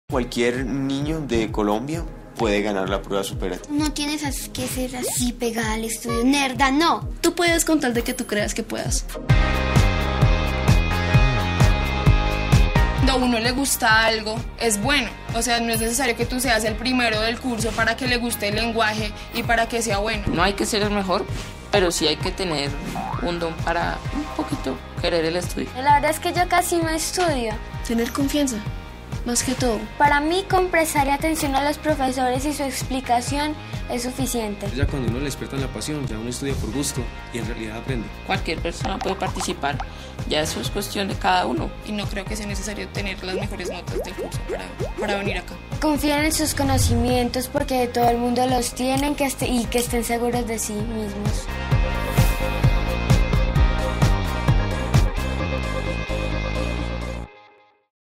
¡No nerd!
No es que seas un Nerd, ¡sólo vuelas más alto! Para ser el mejor, el poder está en ti. Y la oportunidad te la da ¡Supérate con el Saber! Inscríbete ahora.Supérate con el Saber 2014 Supérate Intercolegiados